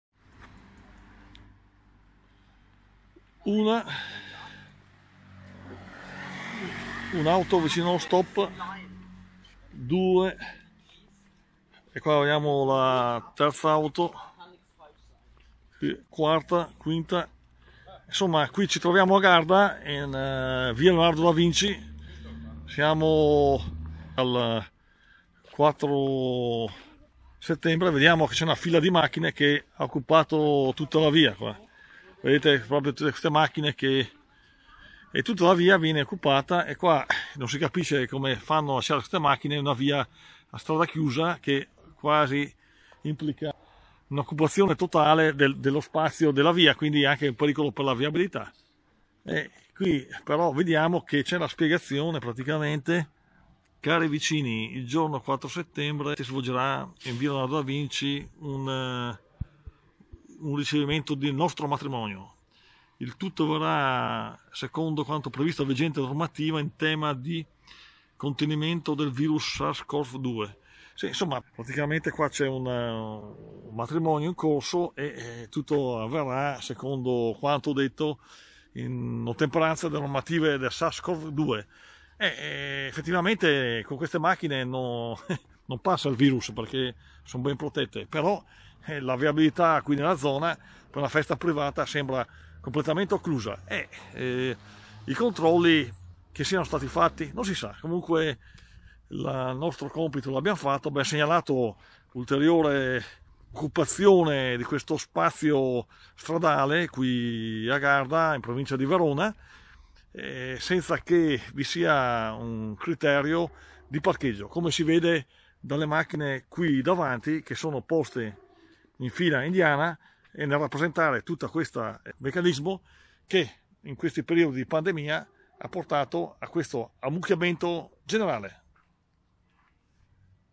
GRANDE AMMUCCHIAMENTO IN VIA DA VINCI
Un incredibile serie di auto "occupa" una via per una festa